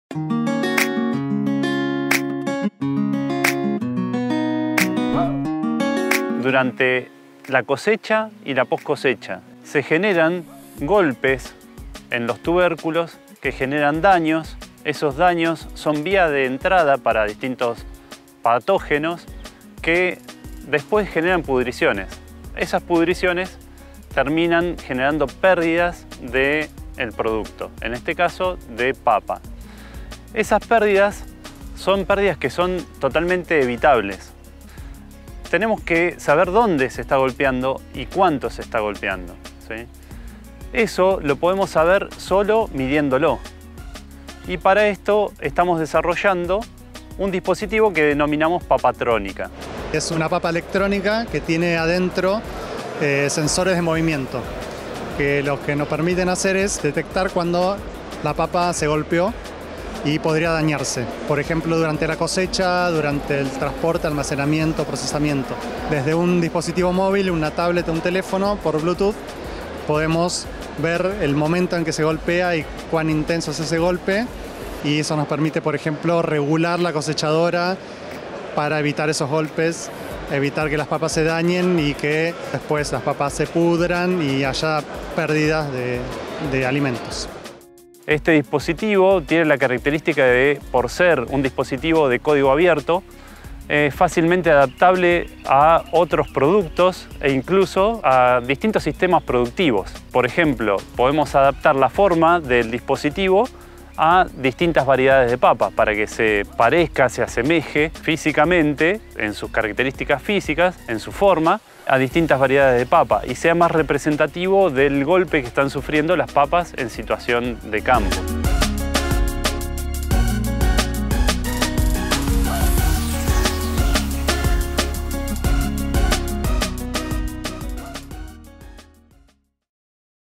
Papatrónica: una AgTech para detectar golpes en la papa
El INTA Balcarce, la Facultad de Ciencias Agrarias de Balcarce y el Centro Universitario de Innovación Ciudadana de la Facultad de Ciencias Exactas de la UNICEN desarrollaron "papatrónica", un dispositivo de código abierto que permite identificar el lugar y el momento en que se producen golpes en la papa durante el proceso productivo🥔